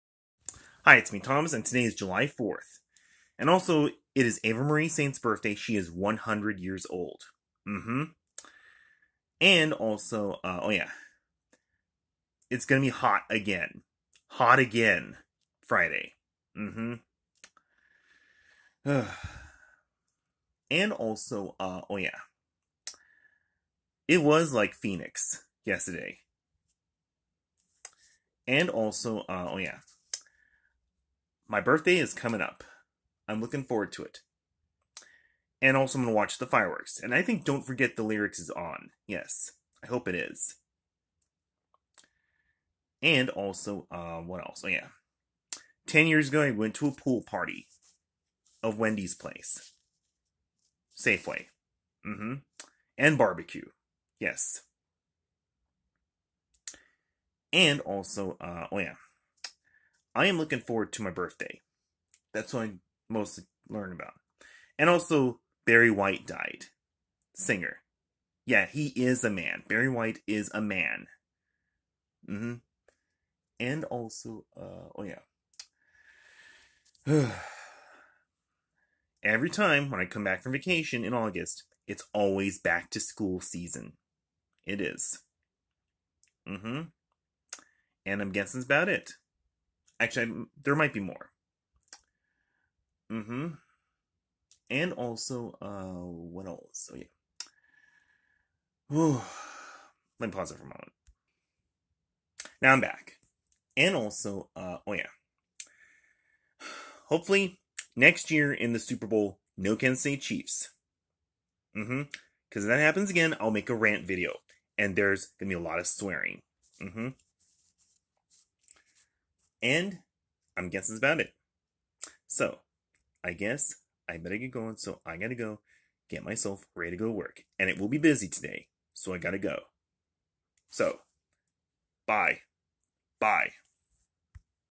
Thomas Murphy knows it is July 4th and eva Marie Saint turns 100 years old. hot again Friday July 5.
Thomas Murphy knows it is July 4th and eva Marie Saint turns 100 years old today. Barry White died and he was a man. 10 years ago Thomas went to a pool party and BBQ from Wendy Safeway. Hot again Friday July 5. Thomas is looking forward to his birthday July 29. Don't forget the lyrics is on tonight. It will be busy today. Next year super bowl LIX no Kansas city Chiefs because he is tired of them in the super bowl. Next video is July 5.